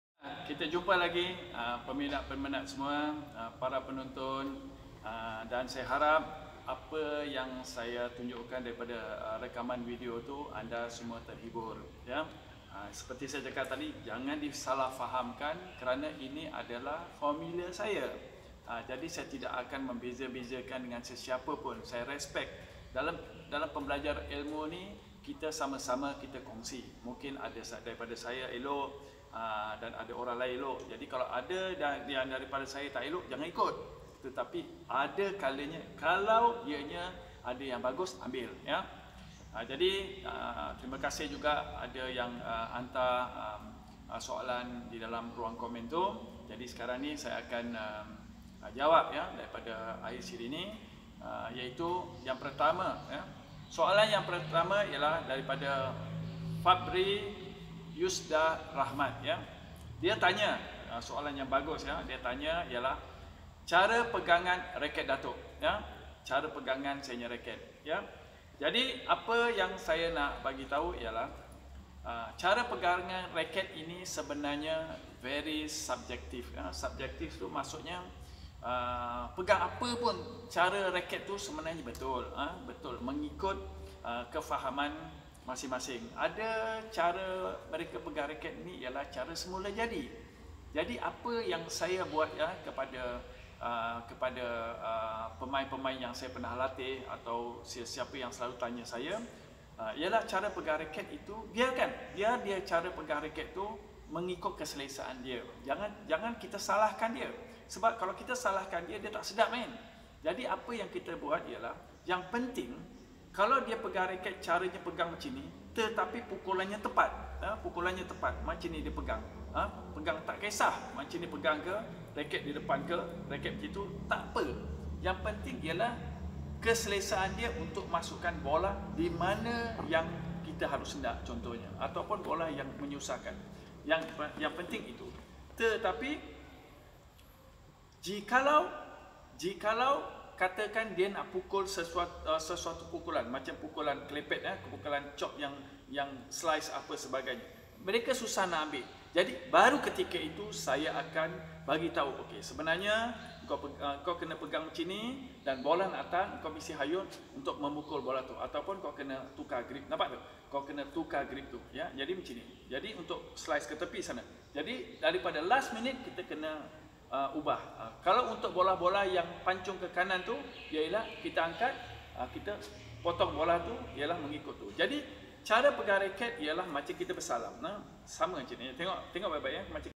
Sisipan Siri 4 Asas Pukulan Atas atas talian bersama Misbun Sidek
Siri ini merupakan salah satu siri dalam Program Badminton Teknik Asas secara virtual yang dipancar secara langsung menerusi FB Page Nusa Mahsuri hari ini.